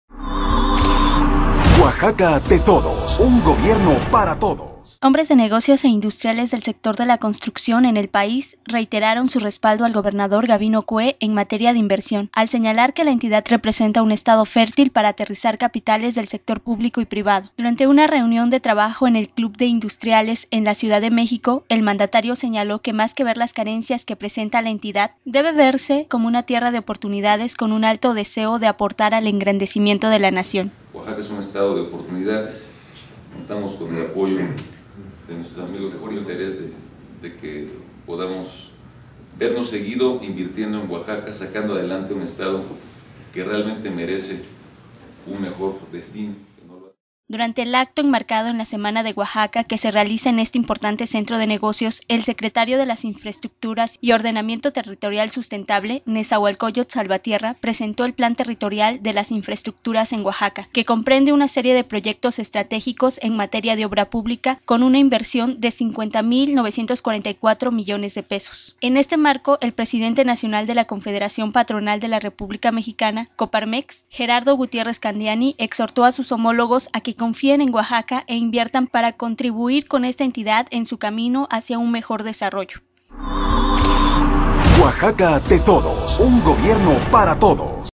Apoyo total de empresarios y Federación al gobierno de Gabino Cué para invertir en Oaxaca
·         Presentan 13 proyectos de Alto Impacto del Plan Territorial de las Infraestructuras en Oaxaca; inversión supera los 50 mil 900 millones de pesos
·         Industriales consideran a la entidad como plaza segura para aterrizar sus capitales

México, D.F.- Hombres de negocios e industriales del sector de la construcción más importantes del país, dirigentes de cámaras empresariales y el Gobierno Federal, a través de las secretarías de  Comunicaciones y Transportes y de Energía, manifestaron la mañana de este lunes su respaldo incondicional al Gobernador Gabino Cué Monteagudo en materia de inversión, al señalar que la entidad representa un estado fértil para aterrizar capitales del sector público y privado.

El mandatario encabezó una reunión de trabajo en el Club Industriales en la capital del país, donde el secretario de las Infraestructuras y Ordenamiento Territorial Sustentable, Netzahualcóyotl Salvatierra López presentó el Plan Territorial de las Infraestructuras en Oaxaca (Platino), con el tema: "Oaxaca, Situación y Perspectivas, Infraestructura y Ordenamiento Territorial", que comprenden una serie de proyectos estratégicos en materia de obra pública, con una inversión de 50 mil 944 millones de pesos.

Durante el acto enmarcado en la "Semana de Oaxaca", que se realiza en este importante centro de negocios, el Ejecutivo estatal asentó que más que ver las carencias que presenta la entidad, debe verse como una tierra de oportunidades con un alto deseo de aportar al engrandecimiento de la Nación.

A diferencia del pasado, Cué Monteagudo enfatizó que el suyo es un gobierno con una alta responsabilidad de sumar, poner orden y sacar adelante a Oaxaca, y para ello, es fundamental la participación de dirigentes empresariales, de la iniciativa privada y del Gobierno del Presidente Felipe Calderón.

En presencia de los subsecretarios de Infraestructura de la Secretaría de Comunicaciones y Transportes y de Energía, Fausto Barajas Cummings y Sergio Martínez Alcocer, respectivamente, así como importantes industriales del ramo de la construcción, vivienda y redes carreteras, el mandatario reconoció que aunque ha habido inversiones en la entidad, hace falta consolidar las obras y programas sociales con rentabilidad social, planeación, transparencia de los recursos públicos y licitación de la obra pública al sector público-privado.

En este sentido, hizo referencia a los 13 Proyectos de Alto Impacto del Programa Platino presentados en este encuentro, entre los que destacan: Proyecto Paso Ancho; Proyecto Paso de la Reyna; la vía Mitla-Tehuantepec; la supercarretera Barranca Larga-Ventanilla; el Libramiento Sur; el Circuito Interior y Movilidad y Transporte para la Ciudad de Oaxaca y los 22 municipios conurbados.

También la construcción de un penal de alta seguridad al Sur de Oaxaca --el cual es un proyecto del Gobierno Federal-, la edificación de cinco polideportivos, rellenos sanitarios, desarrollos habitacionales, un Centro Cultural y de Convenciones y el Proyecto Integral del Istmo de Tehuantepec.

Asimismo, Cué Monteagudo habló de la estrecha coordinación de trabajo y cooperación que su administración mantiene con el Gobierno Federal, lo que representa, dijo, una garantía a quienes deseen invertir en el estado, "porque Oaxaca no está sólo, sino que tenemos el apoyo y el respaldo de la Federación, así nos lo han manifestado y en los hechos lo han demostrado".

Precisó que en  respuesta a esta suma de esfuerzos, el Gobierno del Estado reitera su compromiso de ser un gobierno facilitador, generar todas las condiciones para incentivar a los inversionistas, procurar las condiciones de desarrollo en las regiones y fortalecer la relación institucional con la iniciativa privada, en particular, con el sector de la construcción.

 "Las cosas empiezan a cambiar en Oaxaca, porque emana de un esquema novedoso y aliancista, que genera muchas expectativas y este tipo de ejercicios de participación ciudadana, de transparencia y de orden es lo que queremos dejarle a Oaxaca"

Y Agregó: "Oaxaca es un estado de oportunidades, contamos con el apoyo de nuestros amigos del Gobierno Federal y tenemos el mejor interés de que podamos vernos seguido para seguir invirtiendo en Oaxaca y sacar adelante a un estado que realmente merece un mejor destino".

México, D.F., 6 de junio de 2011